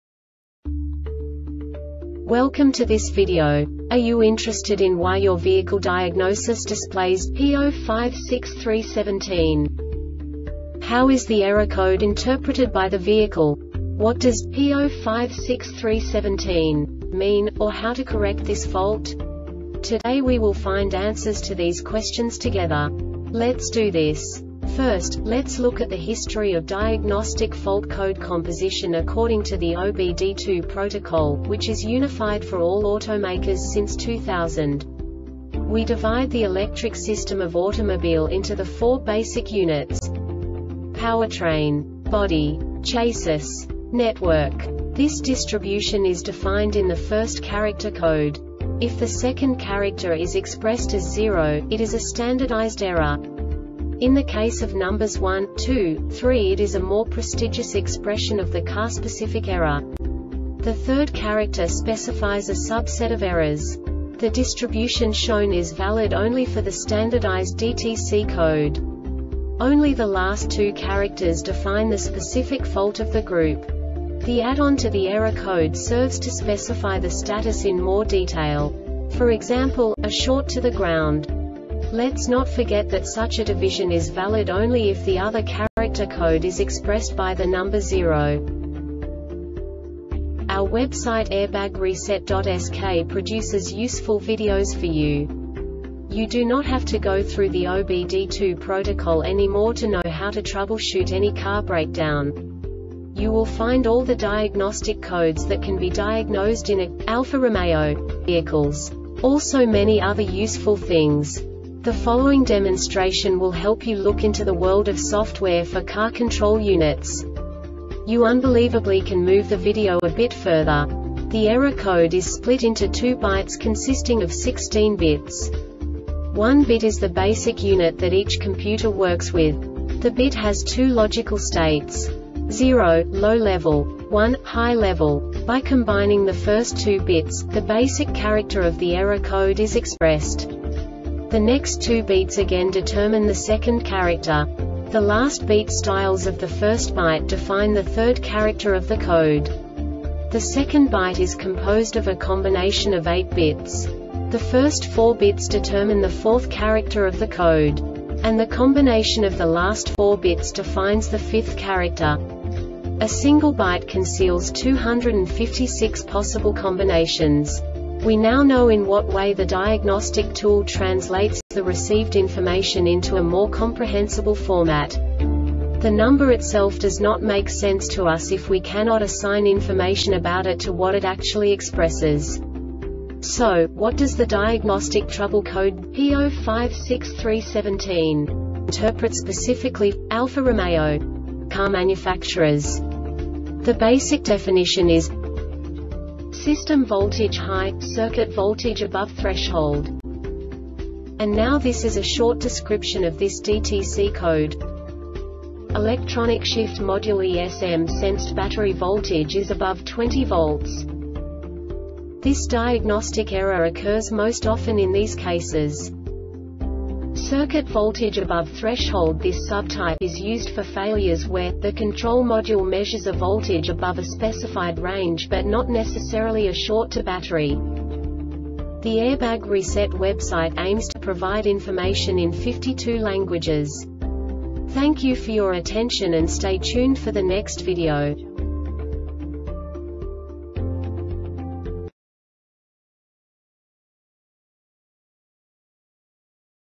DTC Alfa-Romeo P0563-17 Short Explanation
Contents:
0:21 Basic DTC analysis according to OBD2 protocol standard.
1:48 Insight into programming
2:58 A diagnostically understandable description of the Diagnostic Trouble Code
3:05 Basic error definition

Make:
Alfa Romeo
Code:
P0563-17
Definition:
System Voltage High - Circuit Voltage Above Threshold
Description:
Electronic Shift Module (ESM) sensed battery voltage is above 20 volts.